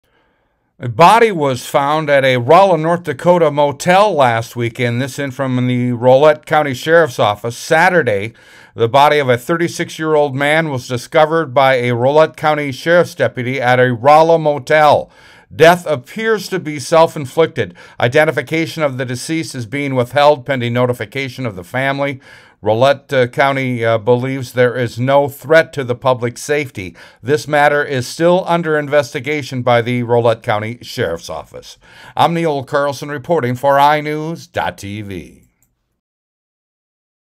Body Found At Rolla, ND Motel
ROLLA, ND (iNewZ.TV) A Deputy discovered a body at a Rolla, North Dakota motel.